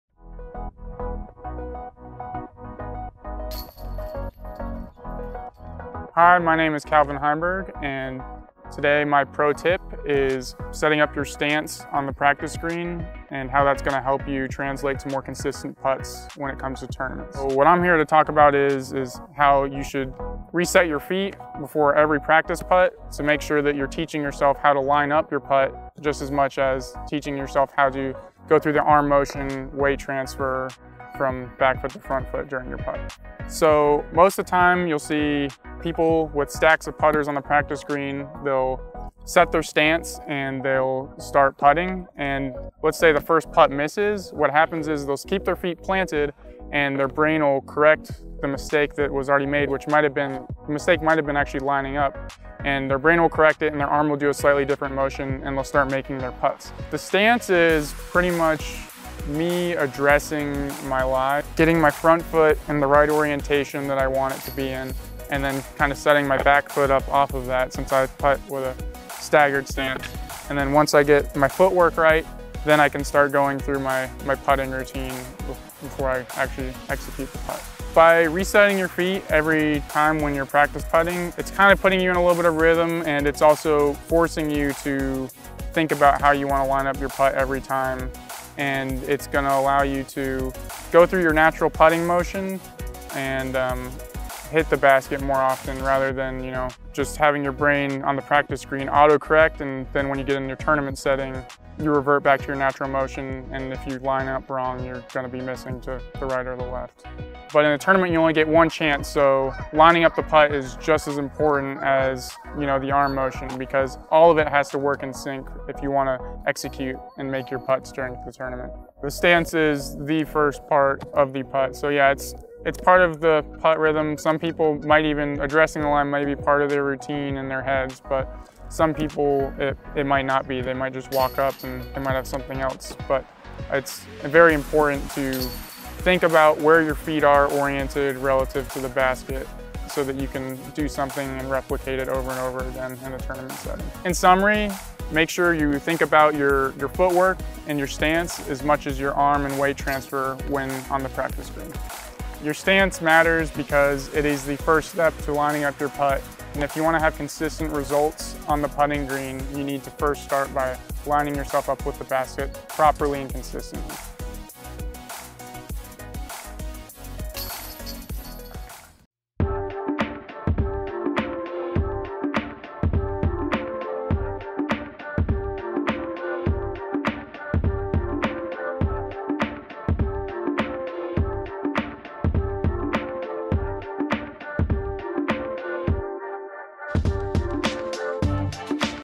Pro Tip: Putting Stance with Calvin Heimburg
Calvin Heimburg, Innova Star Team athlete and 2019 LVC Open Champion, breaks down his success on the putting green—a critical element to low scores in disc golf.

For Calvin consistent putting starts with the first move, the stance. Calvin explains how the stance helps build routine and confidence in putting so you can hit the big putt when the tournament is on the line.

Calvin putts with the KC Aviar. Get yours, along with the full Innova catalog, at the Innova Factory Store